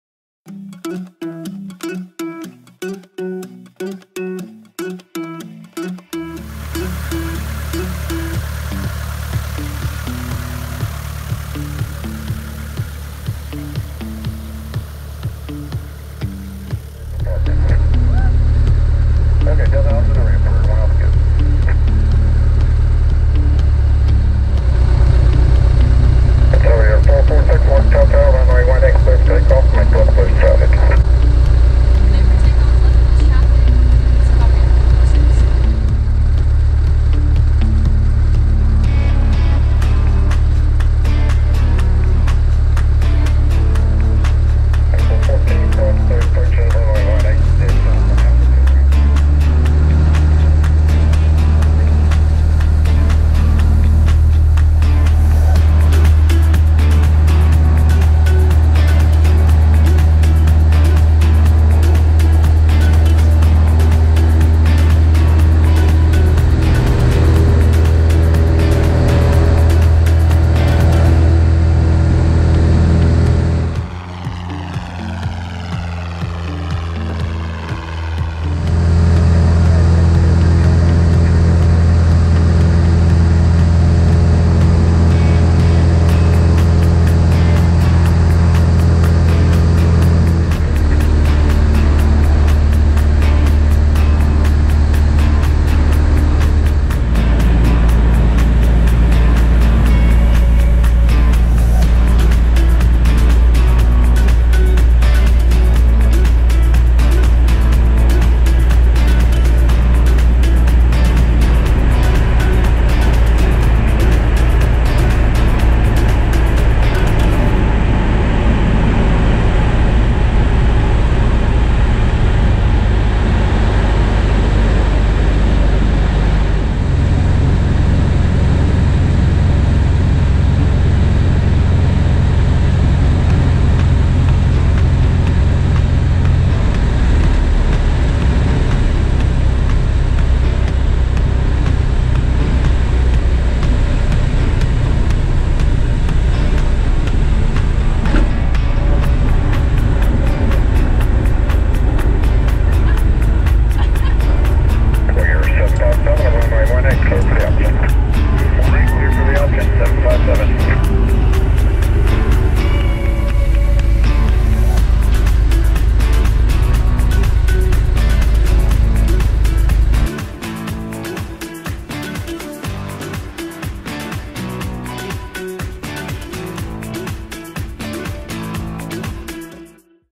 Ellie Gall's very first solo flight in a taildragger
Ellie Gall's very first solo flight in a Bellanca Citabria. September 30, 2018 at KMGW - Morgantown Municipal Hart Field.